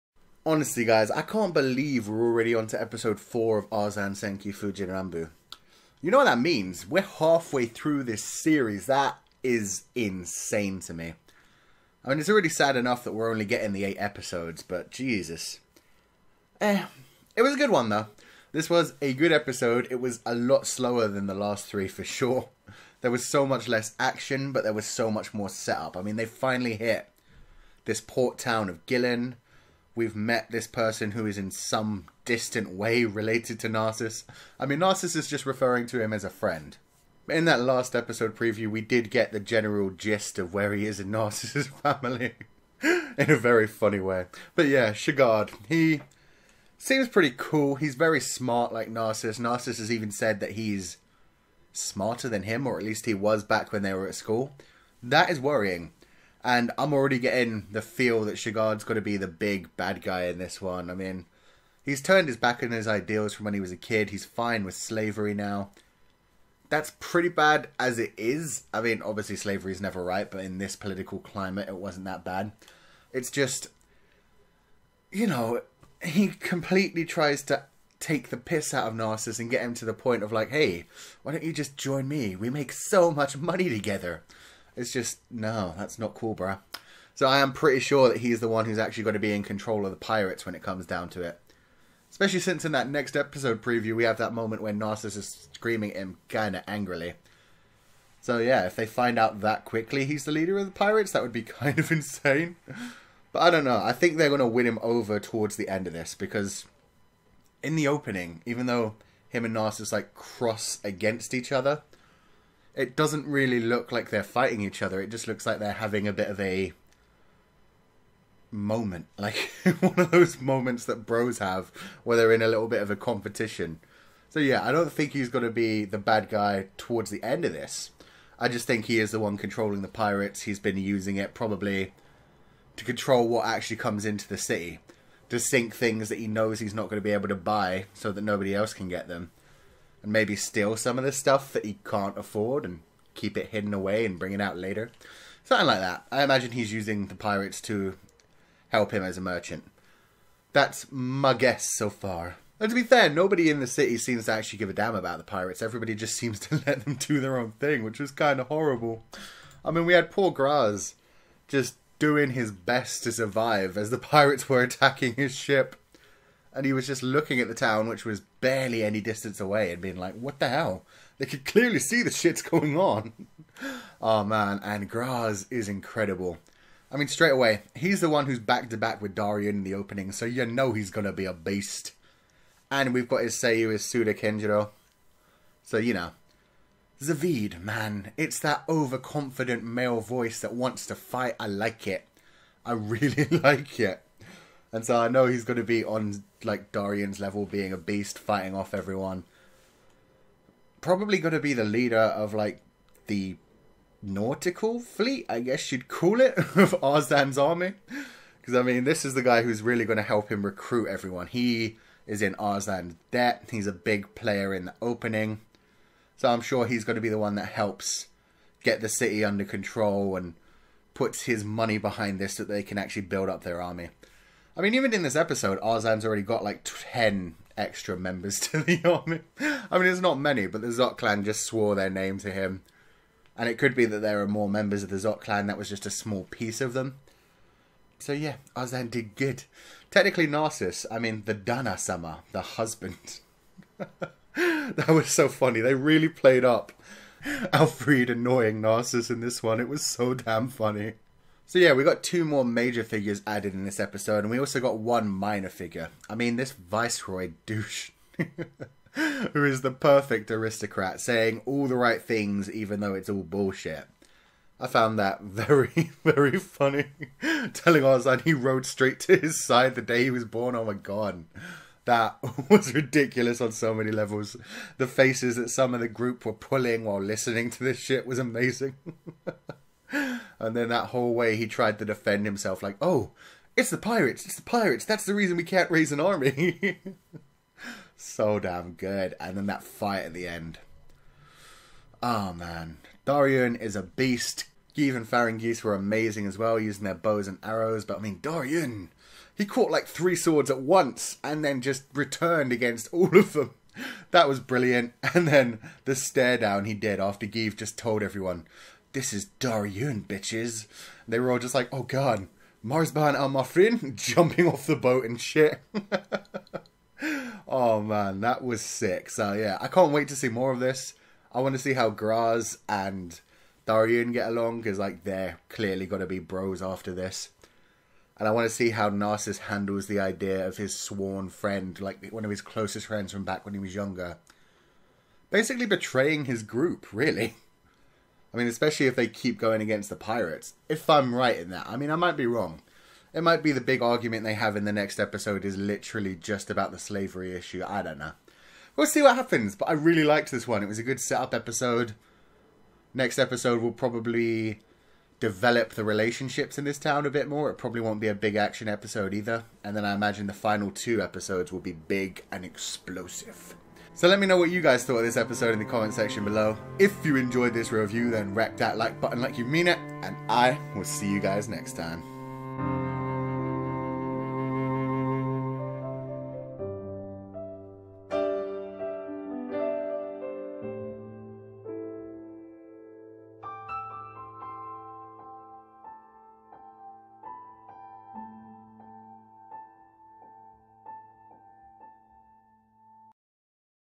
Cunning Smiles. Arslan Senki: Fuujin Ranbu Episode 4 Review
Arslan Senki Fuujin Ranbu episode 4 introduced us to 2 new main characters who seem to be very important to the story. The most interesting of which is Shagard a close friend and distant relative to Narsus who seems to have grown to think very differently from him. His cunning smile and aristocratic attitude make me worried that he may in fact be one of the antagonists of this arc. Grahze on the other hand is a battle ready merchant with the same seiyuu as Zaveid so I can't wait for him and Daryun to become bros on the battlefield!